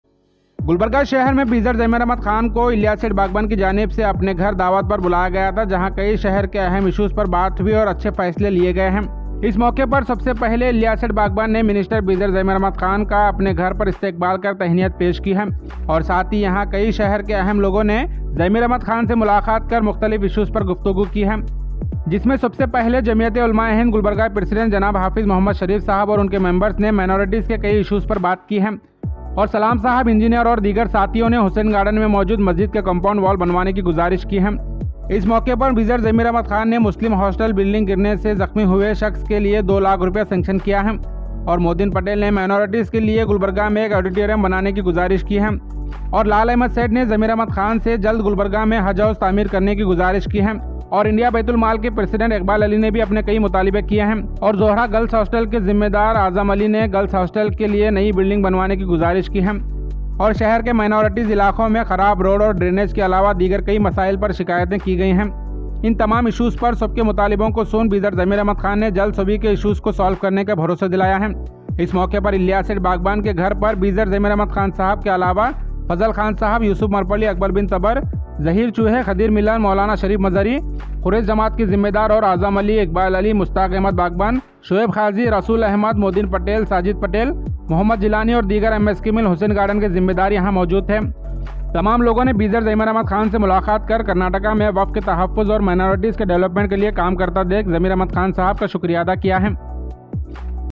Zameer Ahmed Khan visits Ilyas Seth Bagban’s house, addressing multiple minority-related requests...
Gulbarga: Minister Zameer Ahmed Khan visits Ilyas Seth Bagban's house, where many people submitted requests related to minority issues.